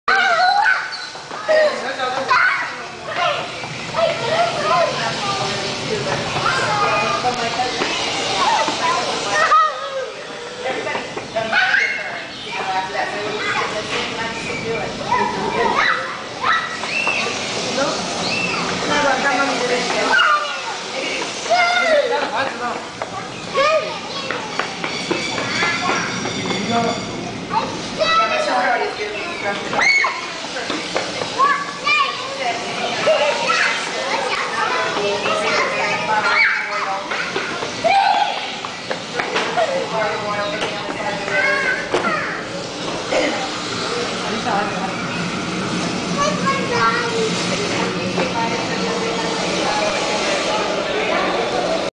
Logan chasing penguin at zoo
He had a blast running back and forth chasing the penguin. I didn't know who to follow, him or the penguin. Priceless.